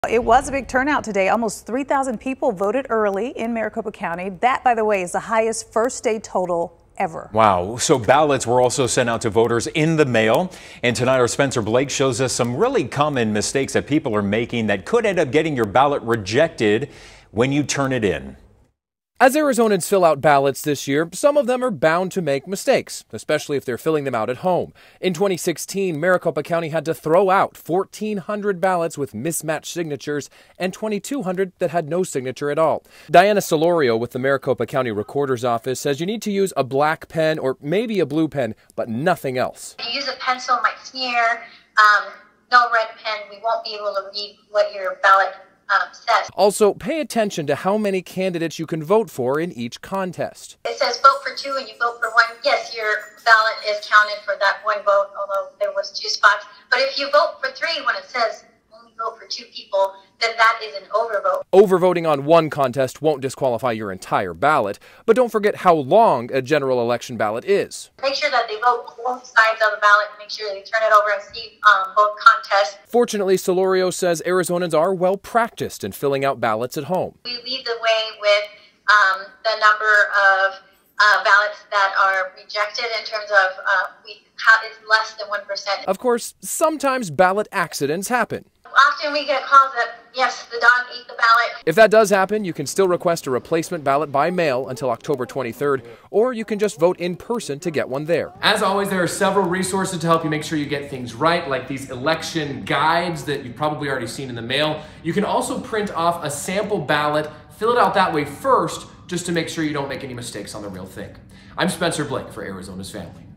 Making sure your vote counts
Avoide these common mistakes when filling out your ballot.